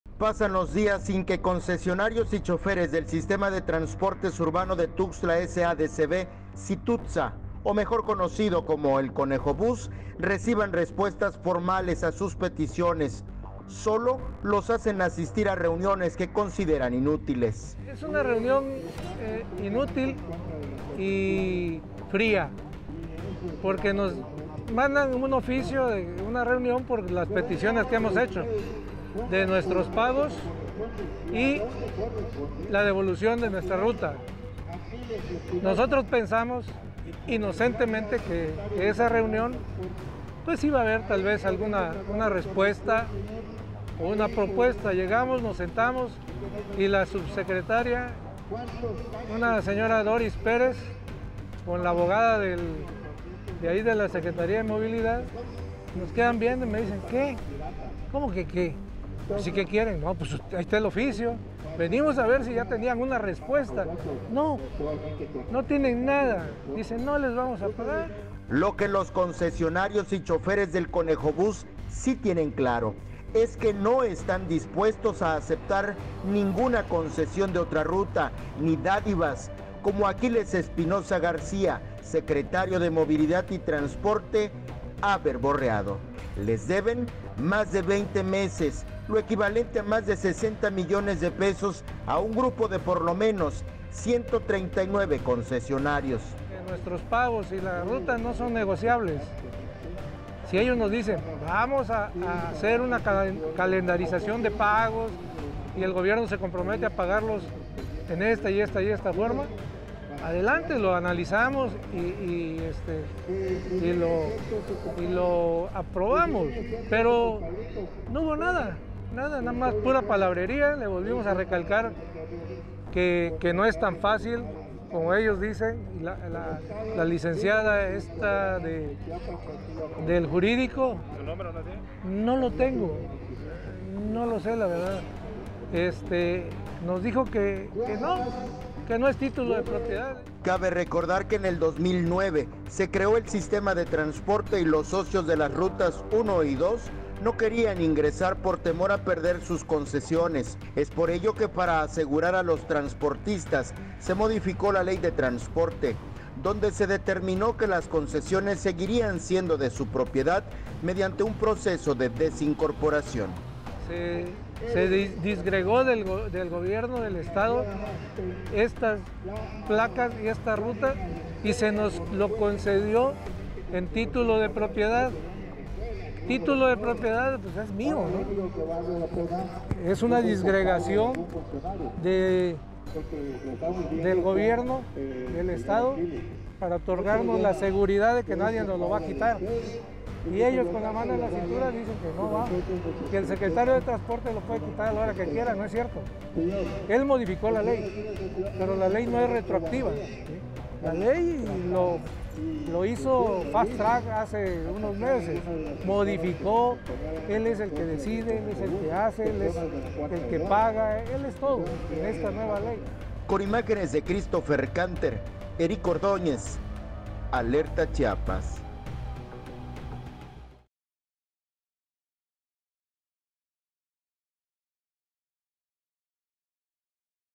Inútil y sin sentido, diálogo con la Secretaría de Transporte
Pasan los días sin que concesionarios y choferes del Sistema de Transportes Urbanos de Tuxtla S.A. de C.V. (Situtsa) o mejor conocido como Conejobús reciban respuestas formales a sus peticiones, solo los hacen asistir a reuniones que consideran inútiles.